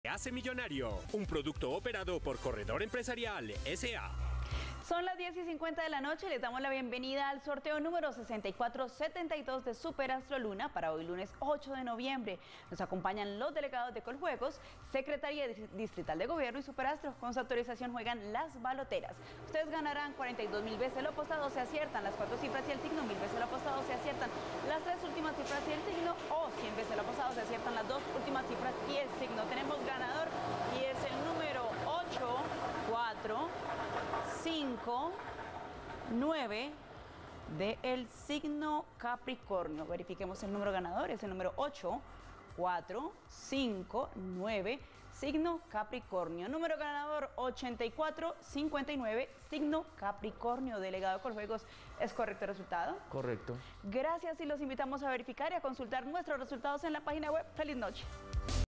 Resultado de ASTRO LUNA del lunes 08 de noviembre de 2021 | SUPER ASTRO ✅🥇🔥💰
Hola buenas noches, te presentamos los resultados de las loterías y chances en Colombia, vamos con el Resultado para SUPER ASTRO LUNA del lunes 08 de noviembre de 2021.

Resultado astro SOL 08 de noviembre
Resultado LUNA 08 de noviembre